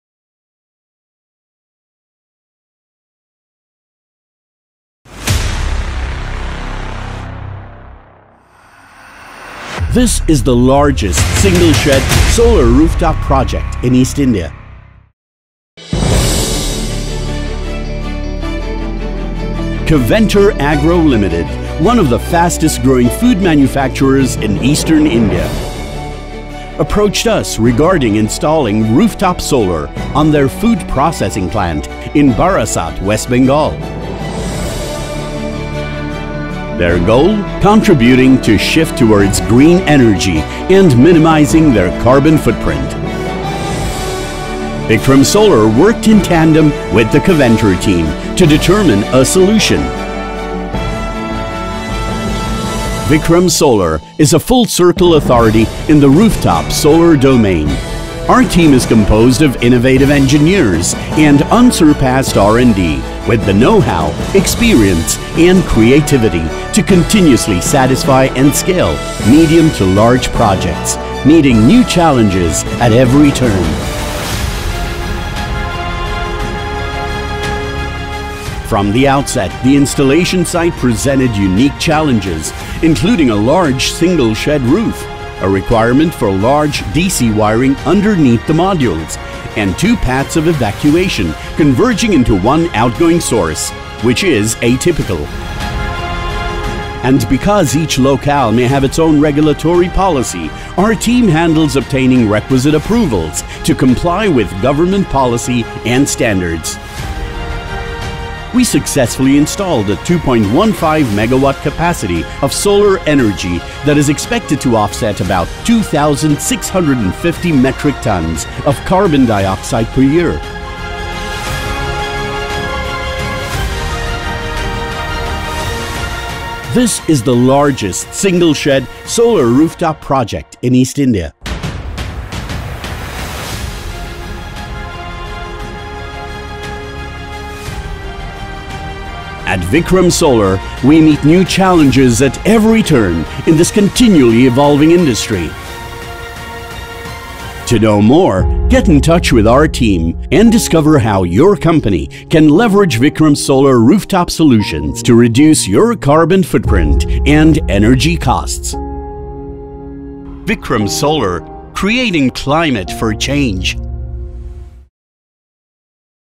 Keventer Agro Limited Solar Project (2.15 MW )
The single shed Keventer Agro Ltd. rooftop solar plant project is expected to have a 2.835 million unit annual energy yield and will offset 2647 metric tonne CO2, annually. About 6240 nos. of Vikram’s high-efficiency Somera Series (monocrystalline) 345 Wp modules were used in the project.